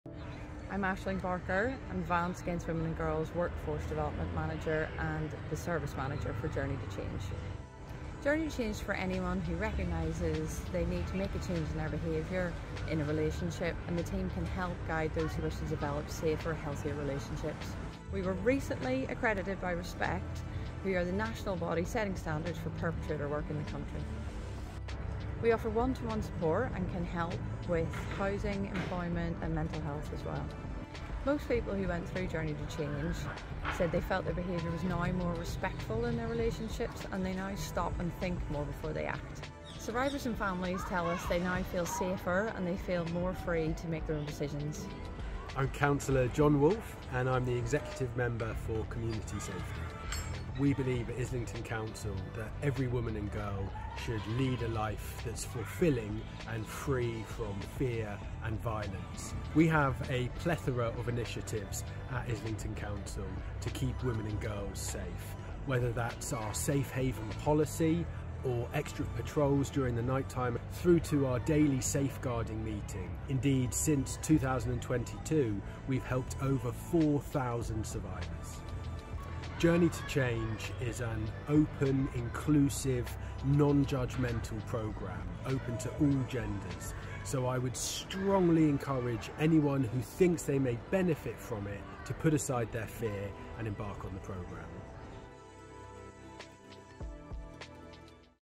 Supporting people who harm their loved ones to change
Domestic abuse has no place in Islington. Our Journey to Change service is for people who have caused harm to or frightened a loved one and want support to change. It was recently accredited by Respect, the gold standard for work with perpetrators of domestic abuse.

If you are experiencing domestic abuse, or you want help to change your abusive behaviour, check out our services